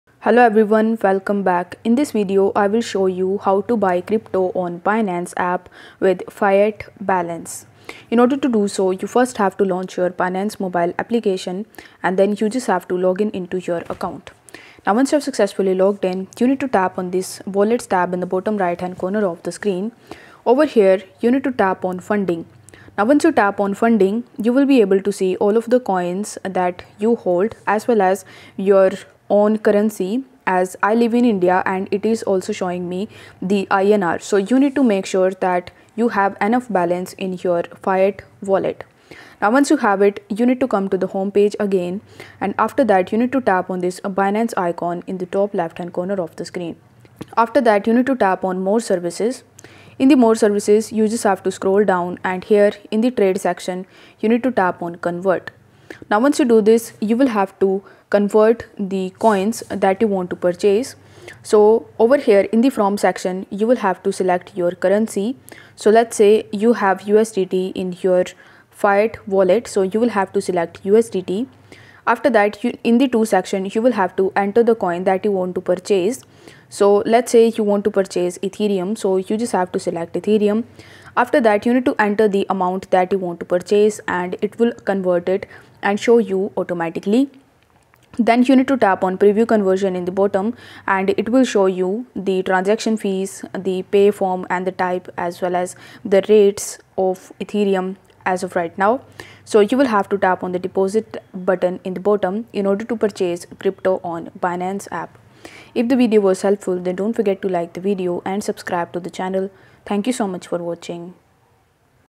How to Buy Crypto on Binance App with Fiat Balance
Welcome to Token Tutorials! In each video, I will show you How to Buy Crypto on Binance App with Fiat Balance.